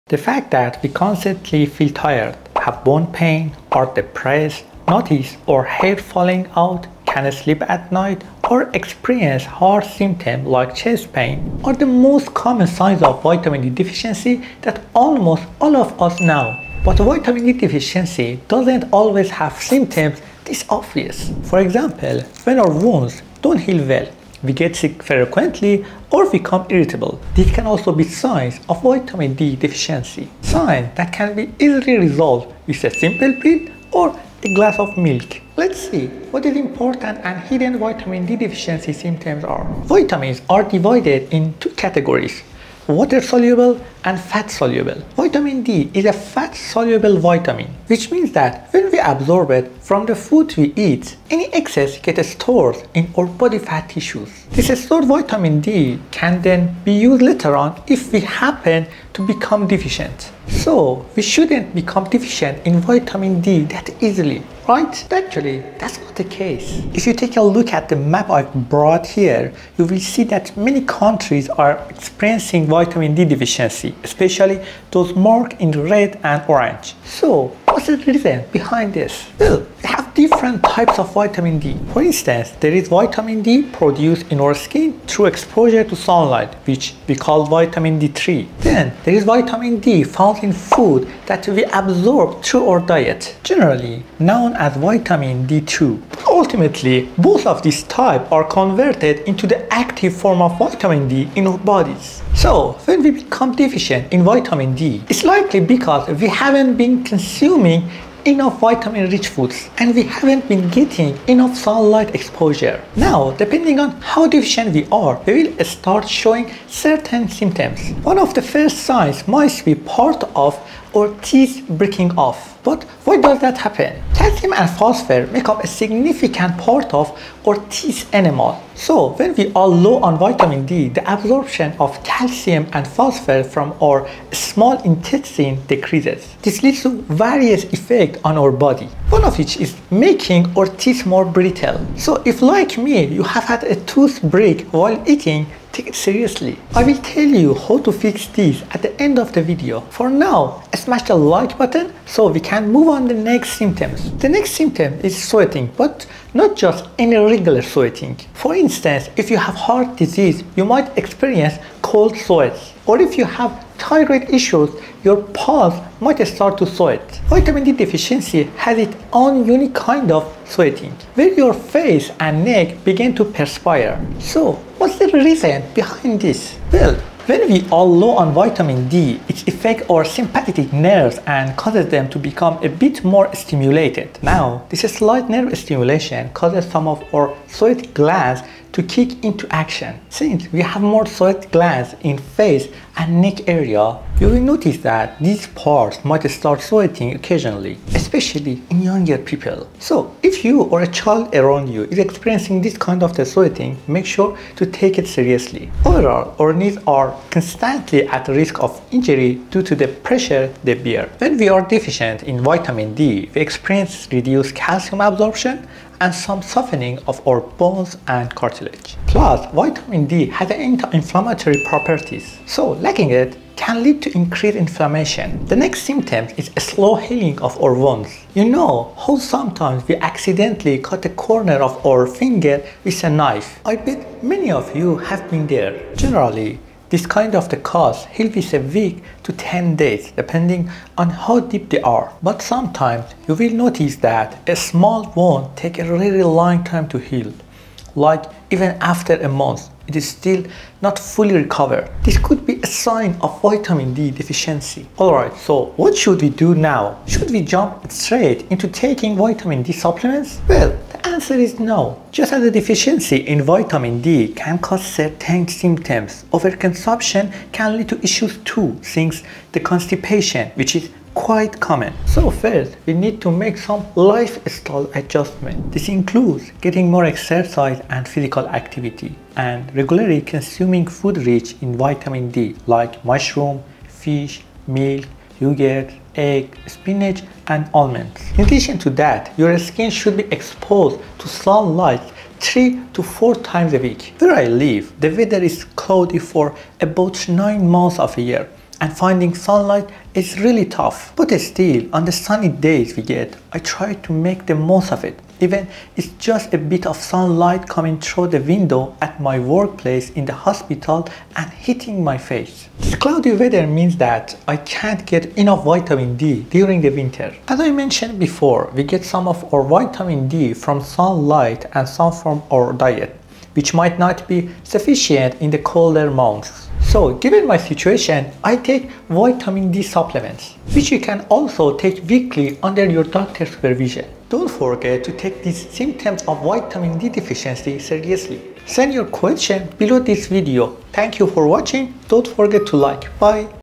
5 Hidden Symptoms of Vitamin D Deficiency You Need to Know
Description:

Are you constantly feeling tired, experiencing bone pain, battling depression, or noticing your hair falling out? These are just the tip of the iceberg when it comes to Vitamin D deficiency symptoms! 🧐 But did you know that there are hidden signs that might be affecting your health without you even realizing it?

🔍 In this video, we dive deep into:

Unexpected Signs of Deficiency – From slow wound healing to unusual sweating patterns, discover the less obvious indicators that your body might be craving more Vitamin D.
The Science Behind Vitamin D – Learn about the different types of Vitamin D (D2 & D3) and how your body stores and utilizes this essential nutrient.
Global Deficiency Insights – See why so many countries are facing Vitamin D shortages and the factors contributing to this widespread issue.
Impact on Teeth and Bones – Understand how a lack of Vitamin D affects your calcium absorption, leading to brittle teeth and knee pain.
Healthy Lifestyle Tips – Find out practical ways to boost your Vitamin D levels naturally through diet, sunlight exposure, and supplements.
🌟 Why You Should Watch:

Educate Yourself: Gain a comprehensive understanding of how Vitamin D impacts your overall health.
Identify Symptoms: Learn to recognize both common and hidden signs of deficiency to take proactive steps.
Improve Your Health: Discover actionable tips to enhance your Vitamin D levels and improve your well-being.
👍 Don't Forget to:

📚 Resources Mentioned:

[Link to Vitamin D-rich Foods]
[Recommended Supplements]
[Further Reading on Vitamin D and Health]

Stay Healthy and Informed! 💪✨

00:25 – Common Symptoms of Vitamin D Deficiency
00:40 – Hidden Symptoms You Might Not Know
01:15 – Types of Vitamin D and How They Work
01:50 – Global Vitamin D Deficiency Statistics
02:30 – Impact on Teeth and Bone Health
03:05 – Unusual Sweating Patterns Explained
03:45 – Knee Pain and Inflammation
04:20 – Slow Wound Healing
04:15 – Lifestyle Adjustments and Dietary Tips
04:30 – Importance of Sunlight Exposure
04:50 – When to Consider Supplements
05:15 – Conclusion and Final Tips